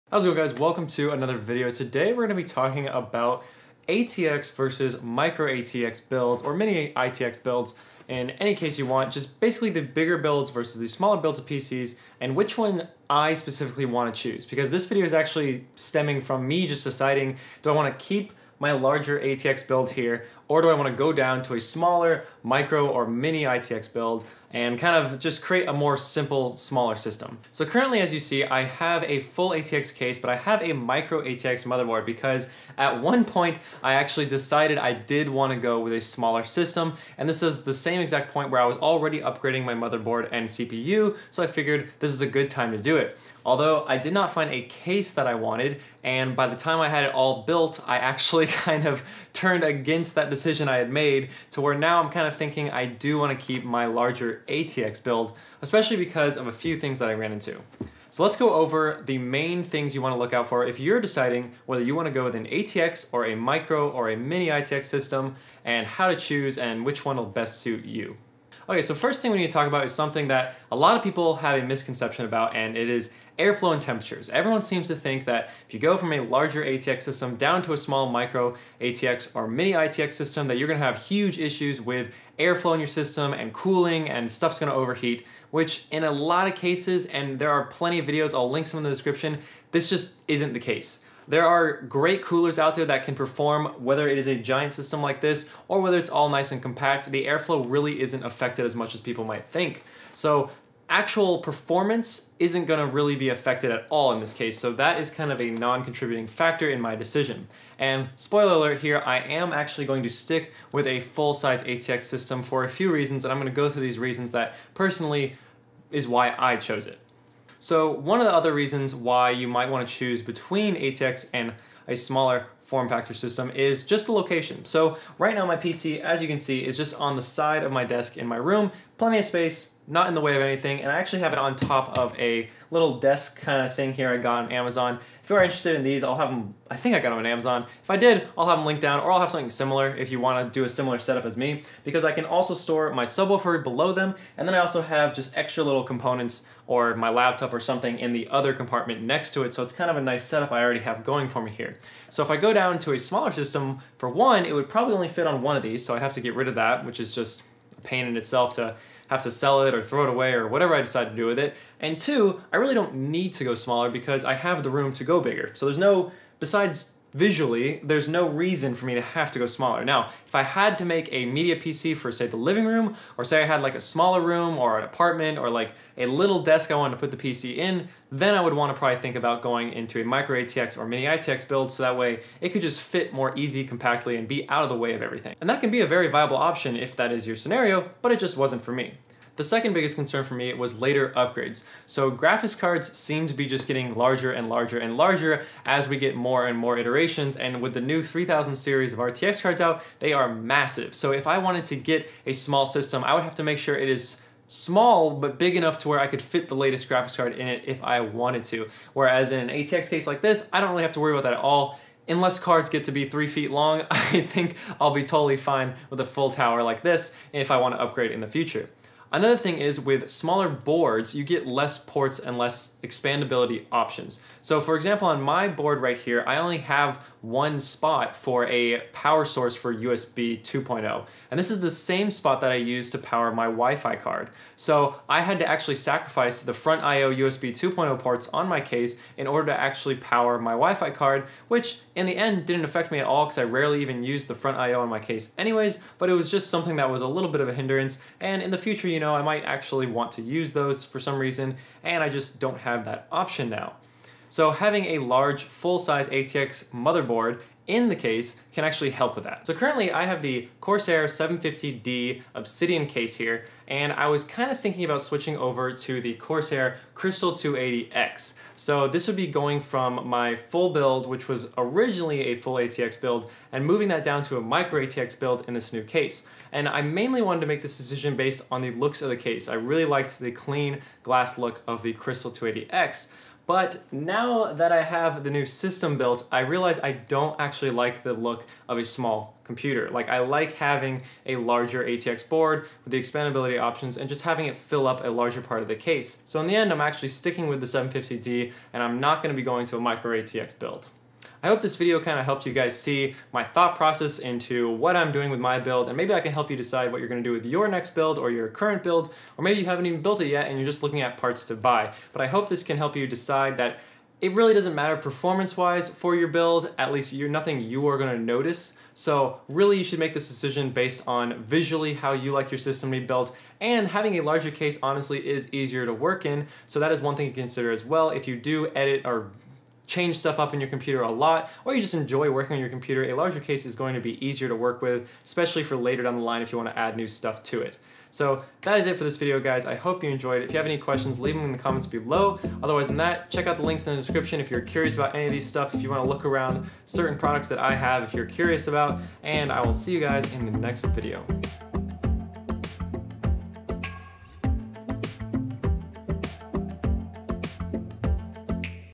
Full Size ATX or Micro-atx PC - Which one to choose?
Made it half way through my transition from an ATX to a micro-atx system and now I'm having doubts...

Corsair Obsidian Case
Corsair Crystal 280X Case
End Table

My Equipment:
Condenser Mic
Lavalier Mic
Gopro HERO 7 Black

"Seek the Lord and his strength; seek his presence continually!" - 1 Chronicles 16:11